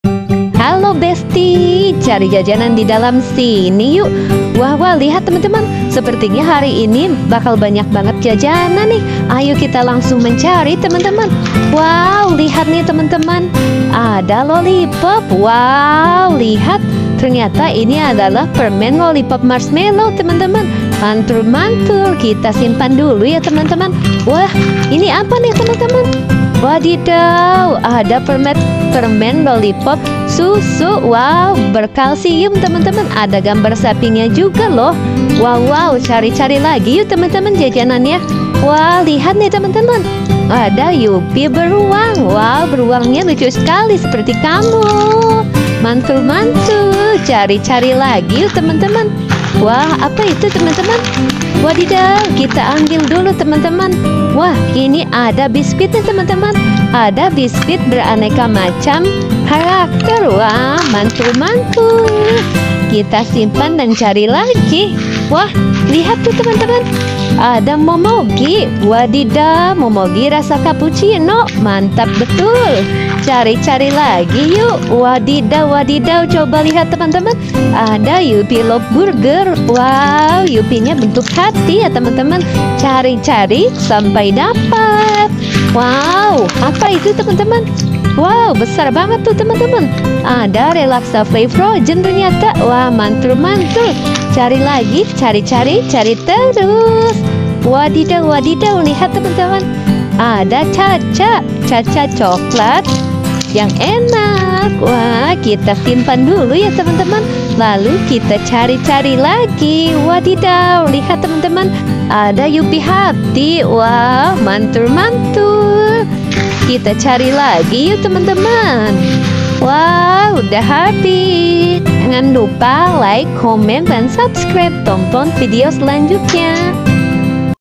Hunting For Viral candy snack Yupi Ice Cream, Barbie, Squid Game, Lollipop, Marshmallow clown Part16
Hello friends, let's look for snack

Don't forget to leave your mark by liking commenting and share❤️